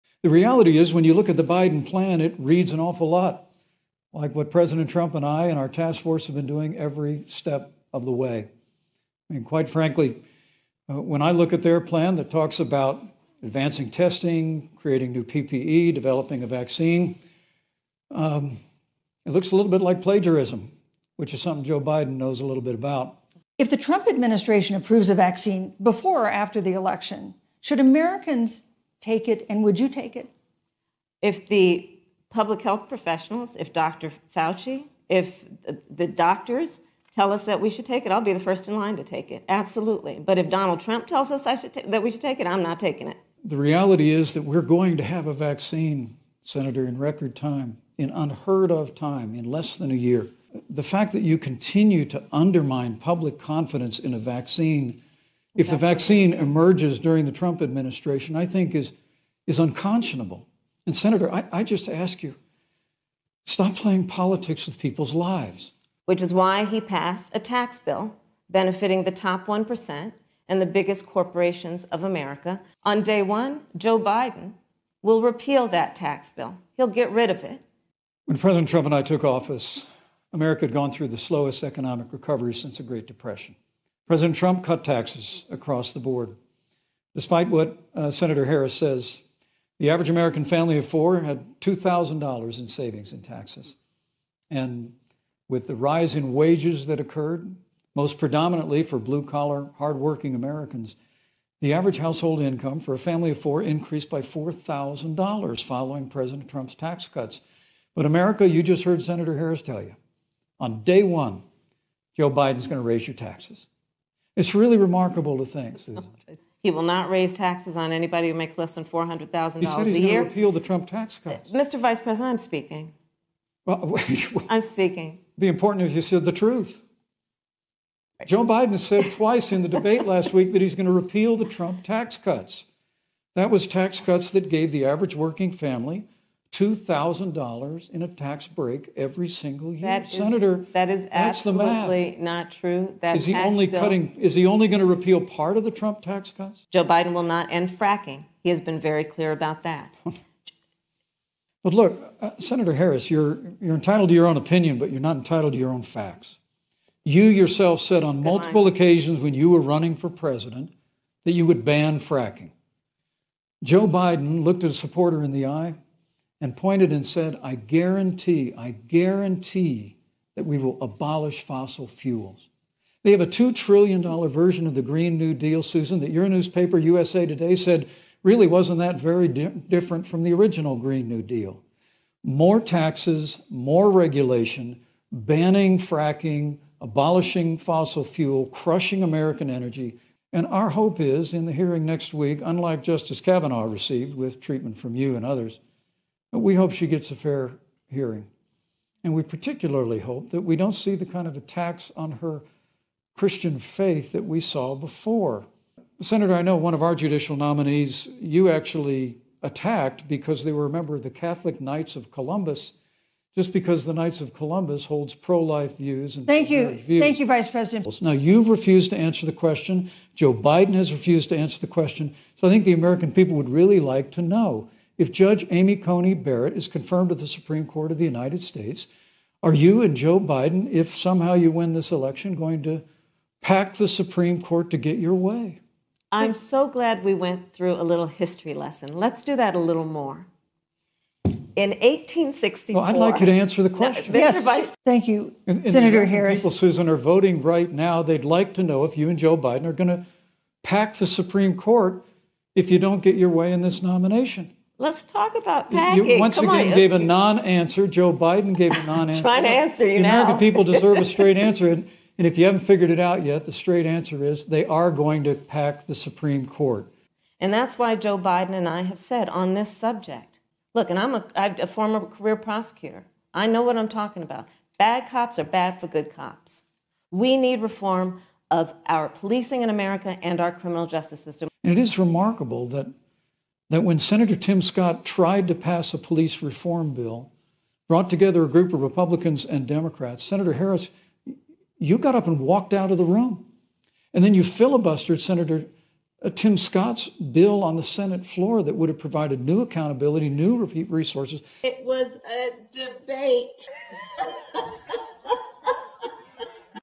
Mike Pence Destroys Kamala Harris Highlights | VP Debate 2020 Highlights
The Best Moments of Mike Pence Destroying Kamala Harris  | VP Debate 2020 Highlights

Unlike the presidential debate last week, this debate was actually enjoyable. But, Mike Pence beat Kamala Harris, without a doubt. I don’t know how anyone could watch this and think that Kamala Harris’ opinions were better than Pence’s Facts. Pence just kept dropping truth bomb after truth bomb, and Harris never stood a chance.

So yes, I think Mike Pence destroyed Kamala Harris in the one and only vice presidential debate 2020. And the way some liberals are fixated on the Fly on Pence's head Shows that, I think.